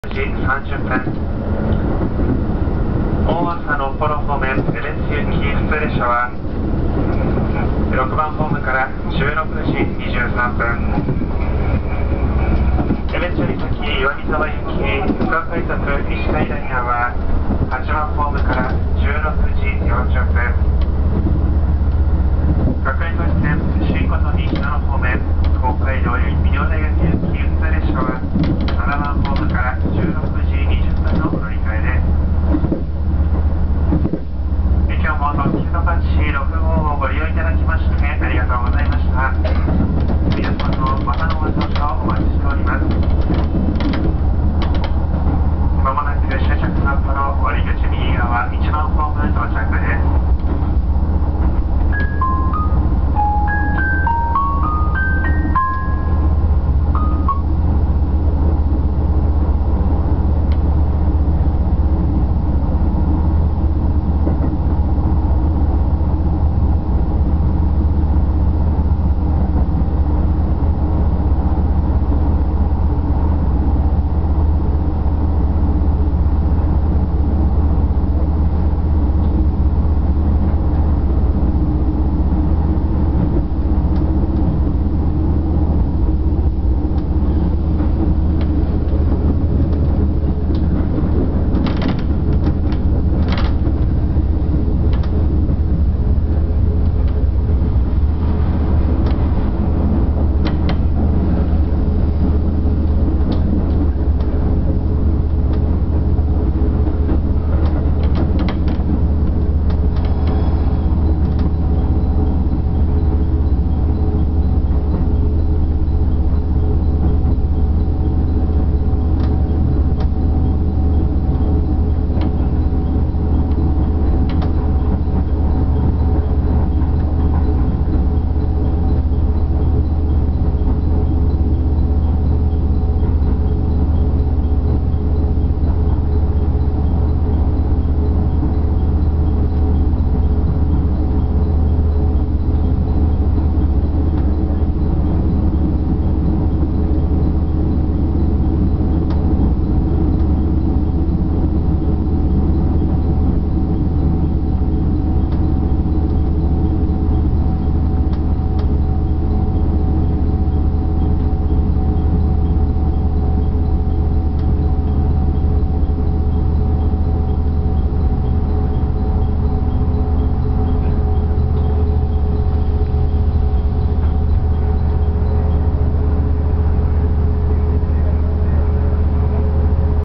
特急とかち6号 前面展望 札幌～新札幌③
札幌入線と同時にカシオペアが発車